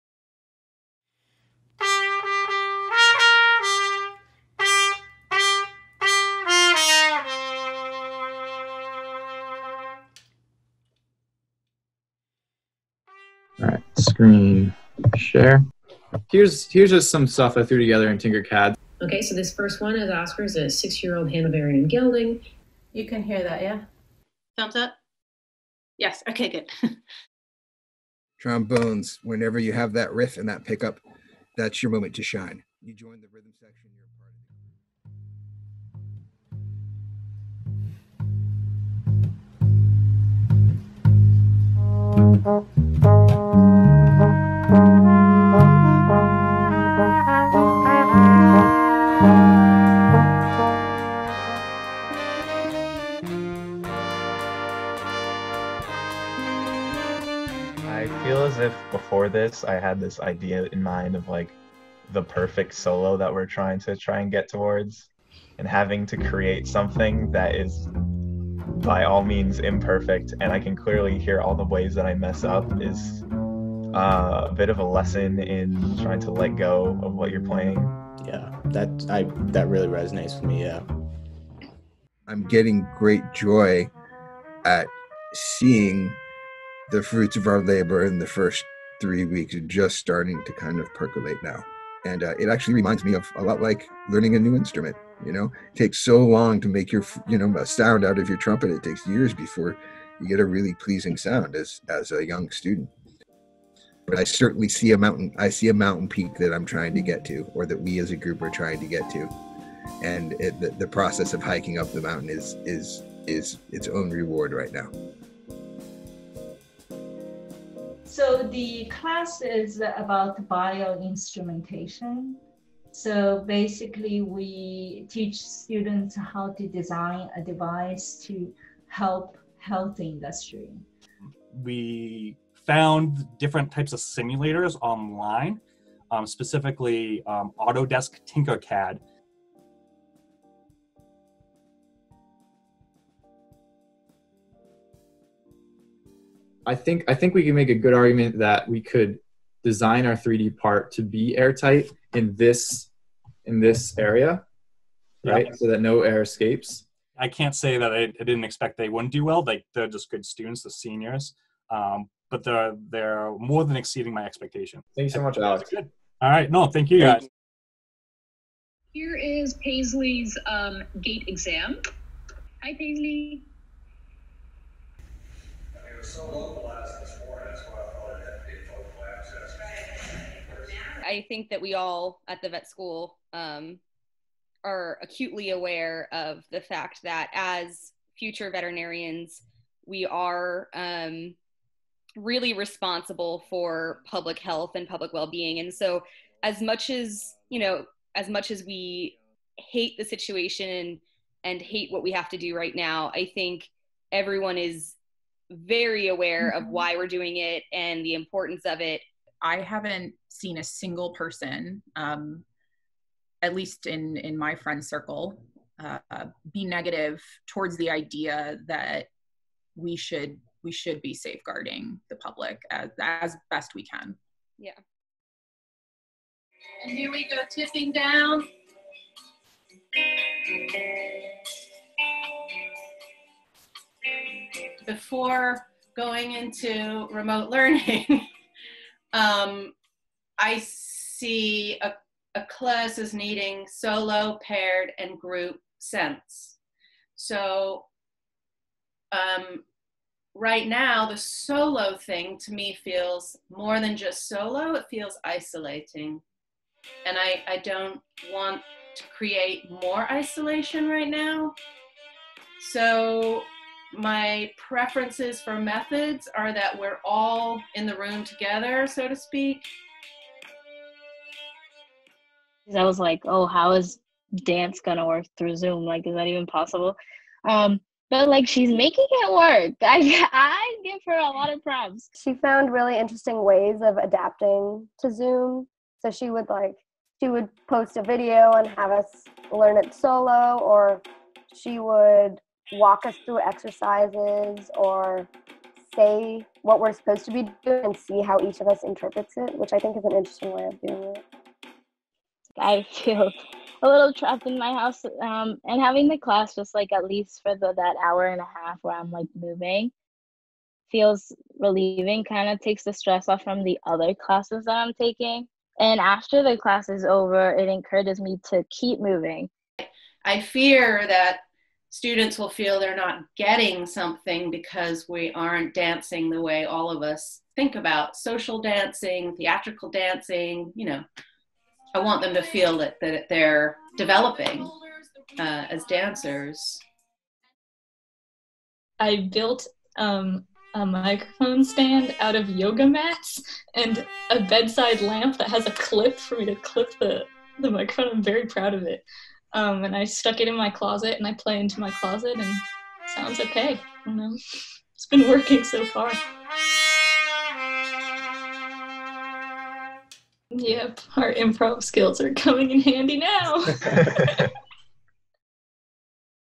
Hands-on learning from a distance
As the radically altered spring semester comes to an end, here are four stories of Cornellians collaborating while isolated.